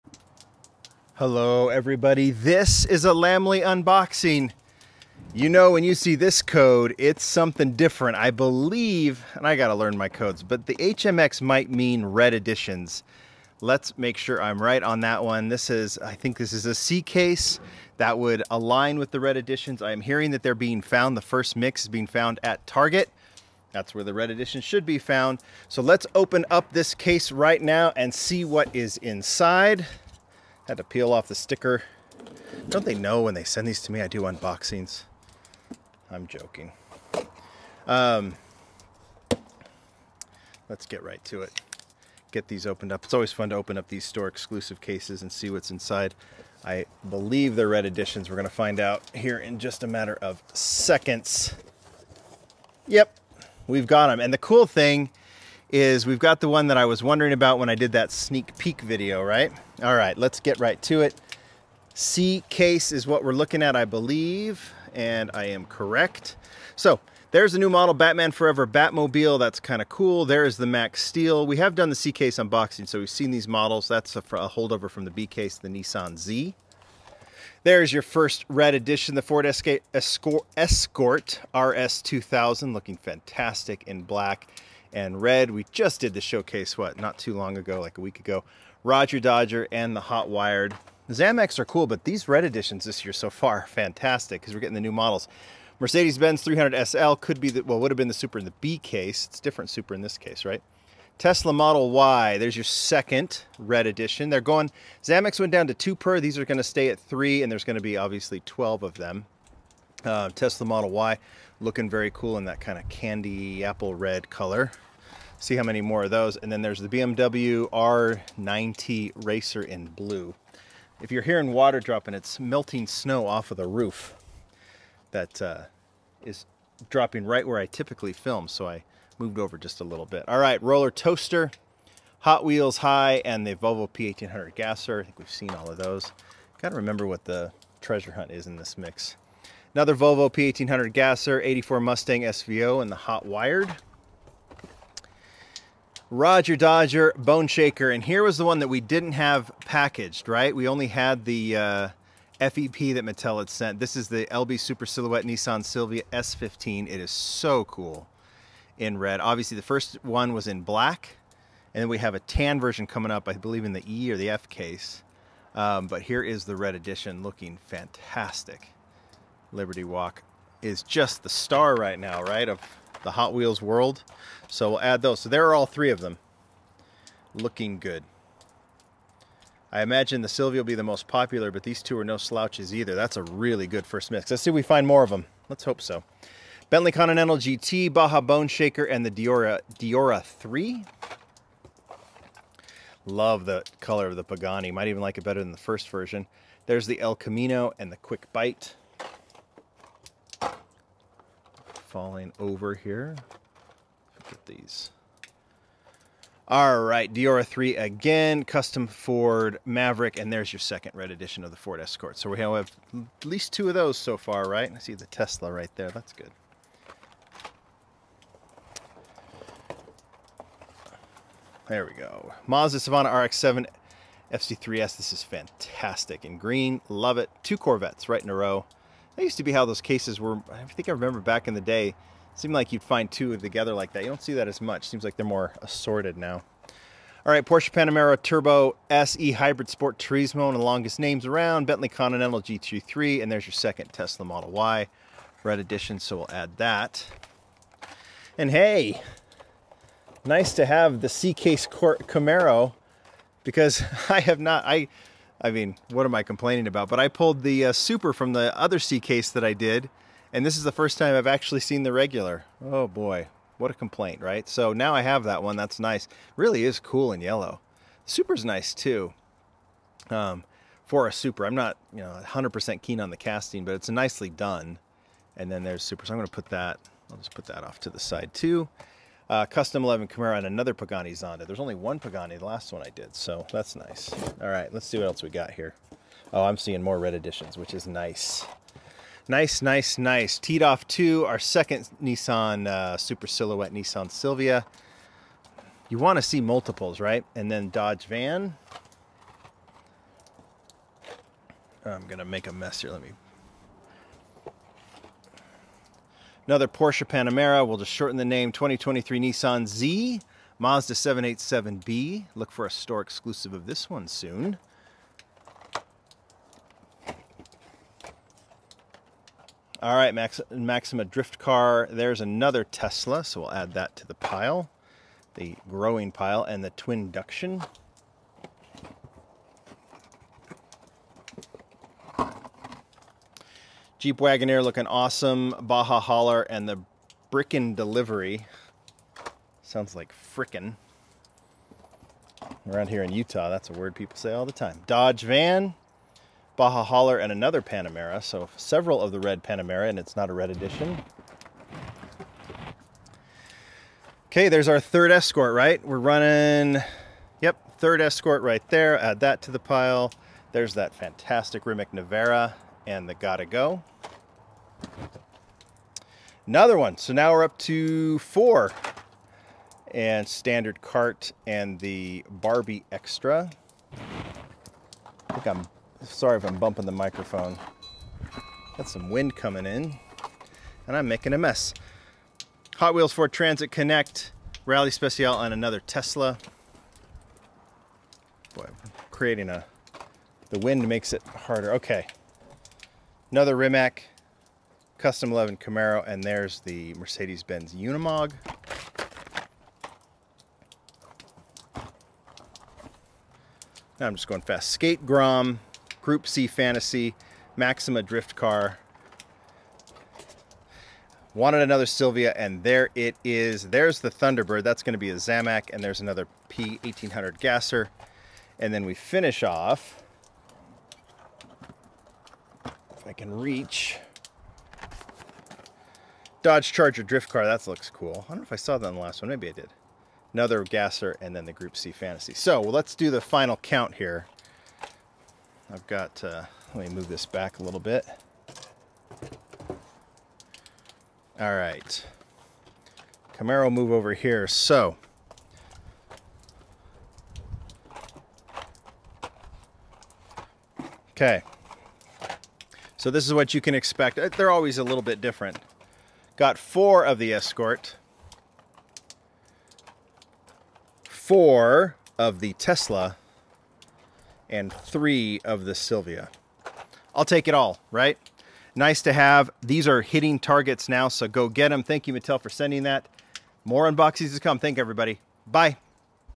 Lamley Unboxing: Hot Wheels 2023 Target Exclusive Red Edition C Case
The Hot Wheels Store Exclusive Mixes are dropping, including the first mix of Target Red Editions.  Let's have a look!